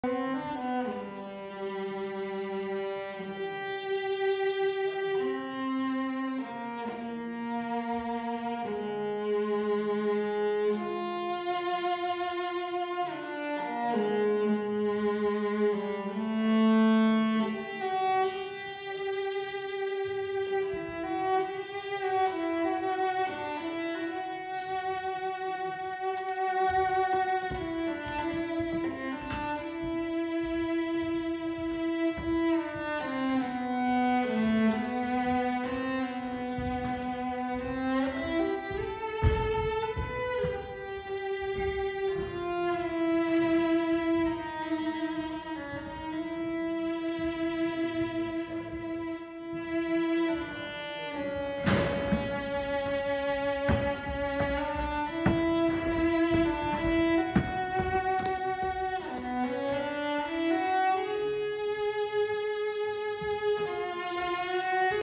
The Loop Science Village Hall
Fine tuning Müller-BBM Vivace RAES - Cello soloist Samuli Örnströmer